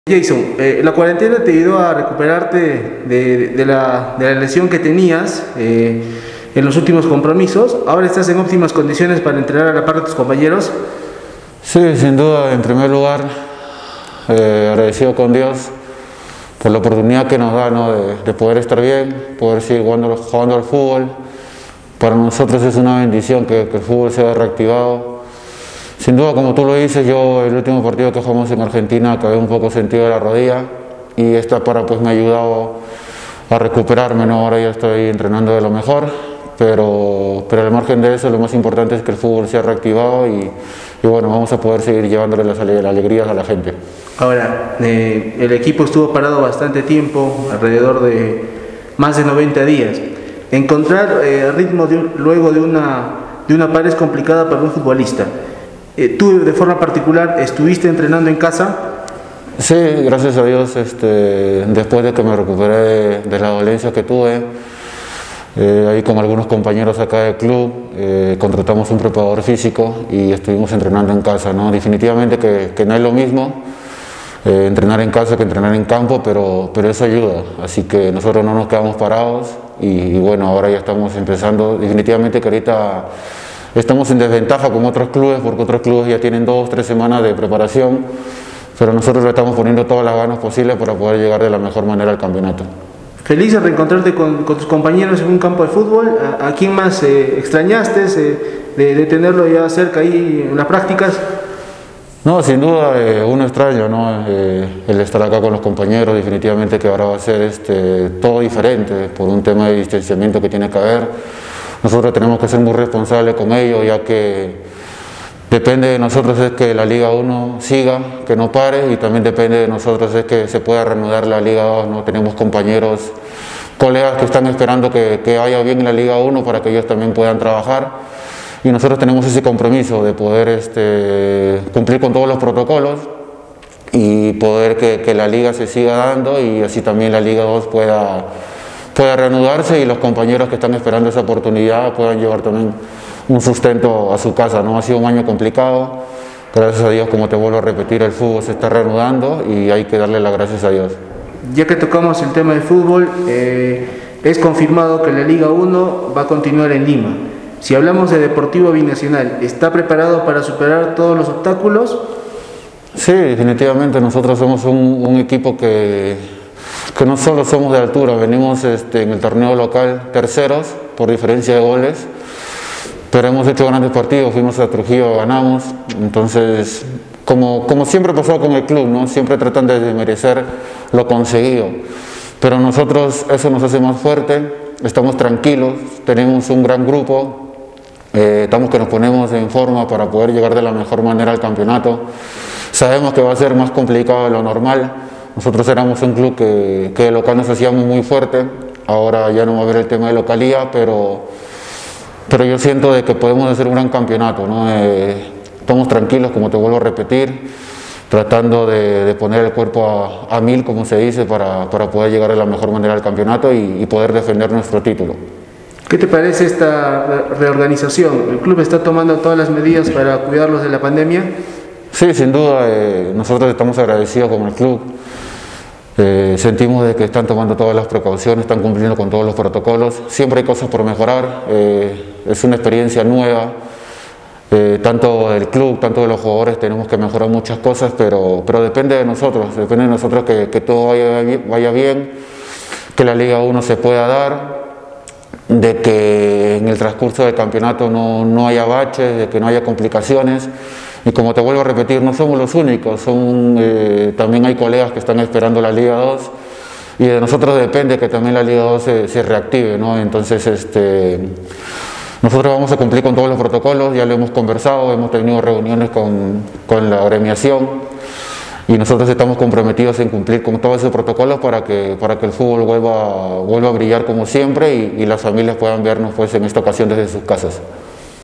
Jeickson Reyes: "Es una bendición que el fútbol sea reactivado"
Jeickson Reyes, defensor de Deportivo Binacional, habló sobre la importancia de la vuelta a los entrenamientos del vigente campeón de la Liga1 Movistar. Además también se refirió sobre la lesión que sufrió y lo mucho que extrañaba estar con el equipo.